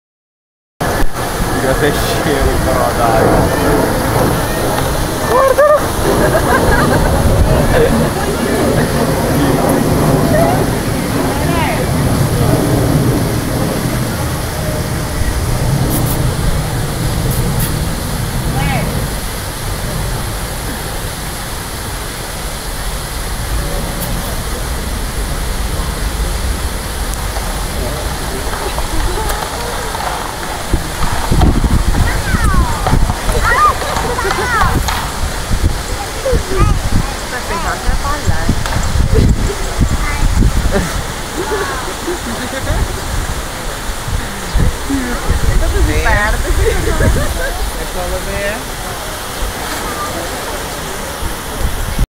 Polar bear playing
Polar bear playing at the Bronx Zoo